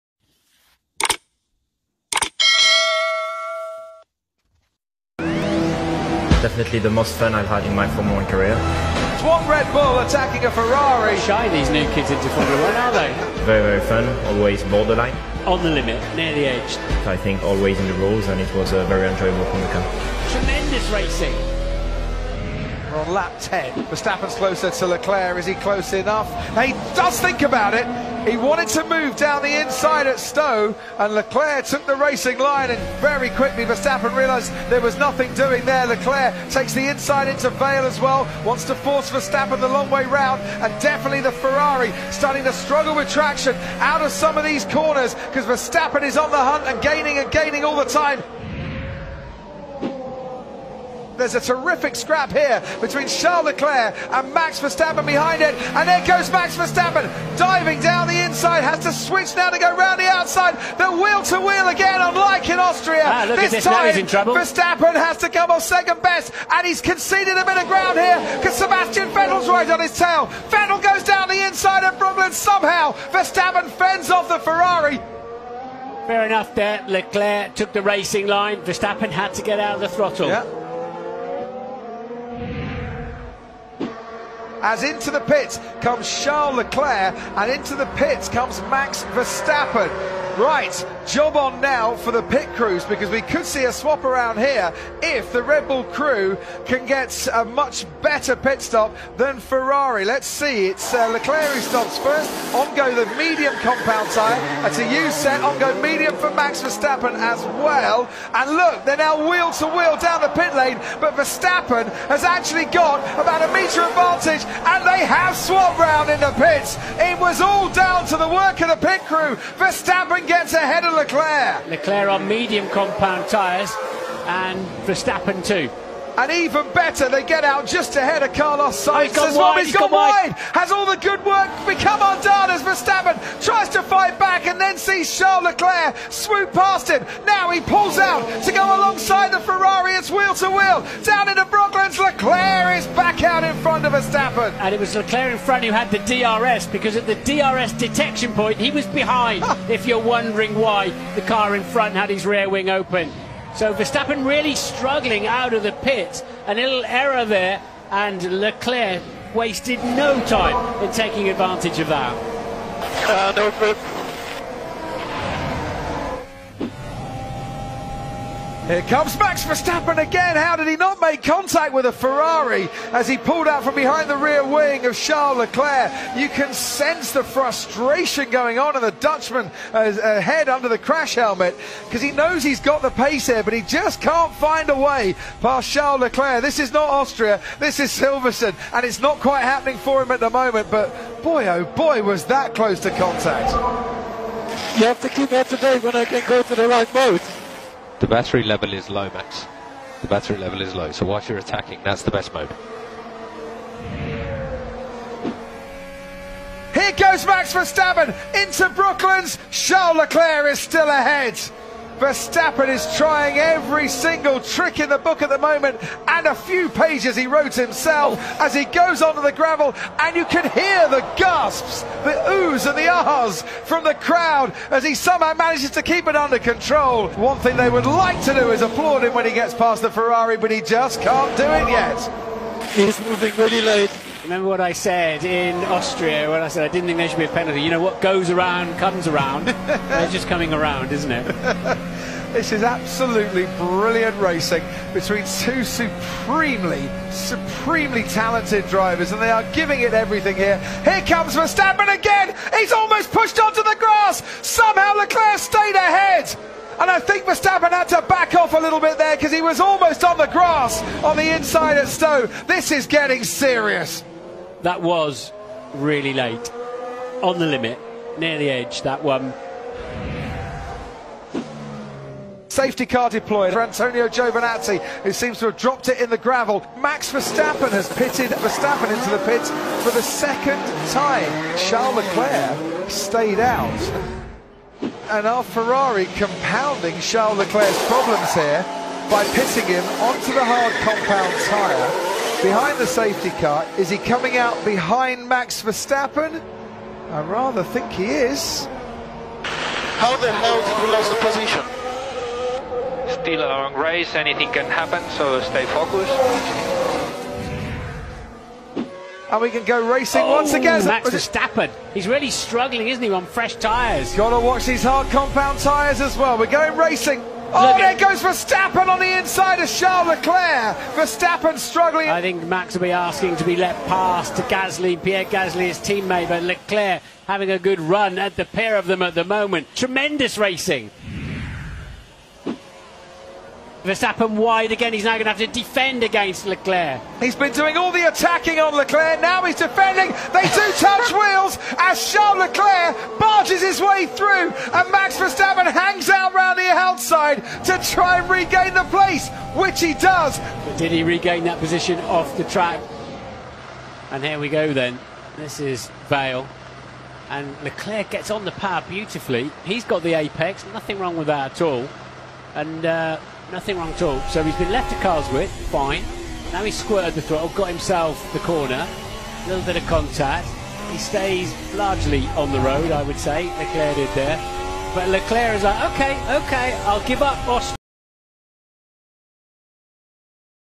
Battle Between Max Verstappen Vs Charles Leclerc || British GP 2019
Leclerc, Verstappen 'extremely happy' with epic battle

Official F1 driver of the day Charles Leclerc said that Sunday's thrilling British Grand Prix was his favourite race of the 31 Grand Prix events that he's taken part in to date, thanks to his extended fight for position with Max Verstappen.
It was something of a reprise of their on-track duel in the Austrian Grand Prix in which Leclerc lost out on victory to a tough-but-fair move by the Red Bull driver during the final laps.